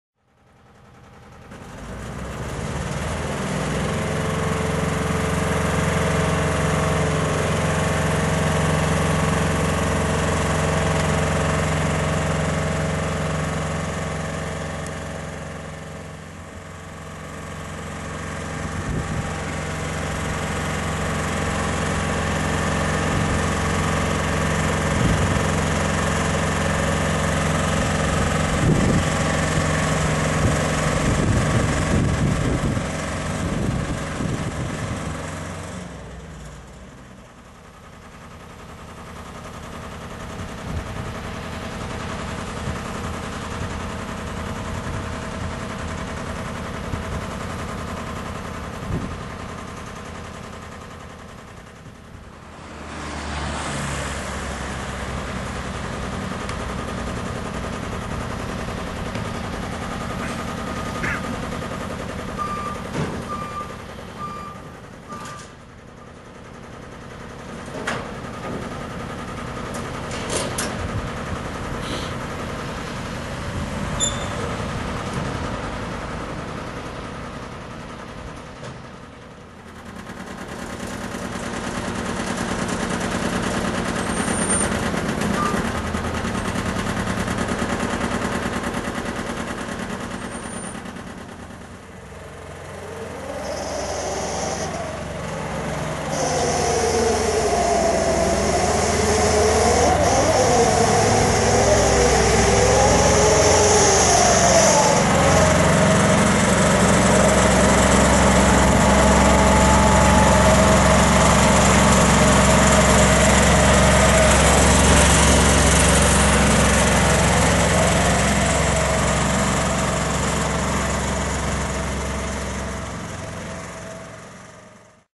Haulotte H15SX, Diesel, 4x4, 15m [PfeiferMachinery.com]
This used Haulotte H15SX is in an excellent technical and optical condition. If you are interested in a used machine, Pfeifer Heavy Machinery BV provides the machine you are looking for.